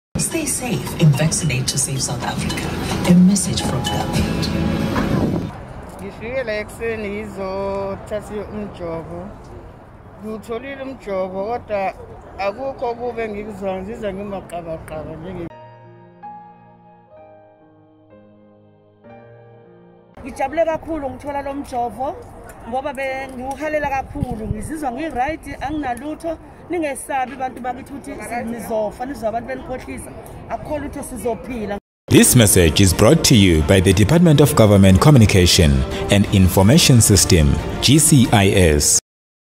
Vaccination Testimonies
Testimony by people who have vaccinated for Covid-19.